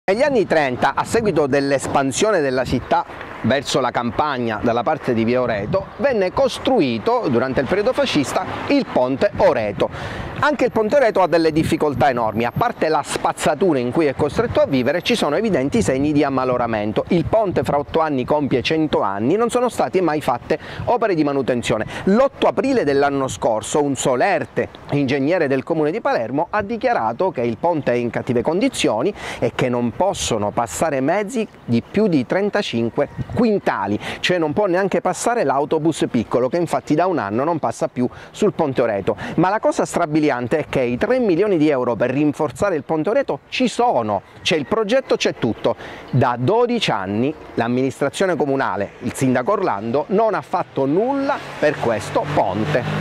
Ponte Oreto da un anno gravi limitazioni. Ci sono 3 milioni per lavori ma prog bloccato da 10 anni
Frutto di un lungo malgoverno dell’amministrazione Orlando.
"Il finanziamento per il consolidamento del ponte risale al 2010 - spiegano Igor Gelarda capogruppo Lega-Prima l'Italia e Giuliana Sgroi referente dipartimento Famiglia Lega Palermo , con quattro milioni di euro di Fondi Cipe già stanziati e tuttora nelle casse del comune. Ma per ben 12 anni tutto è rimasto in un cassetto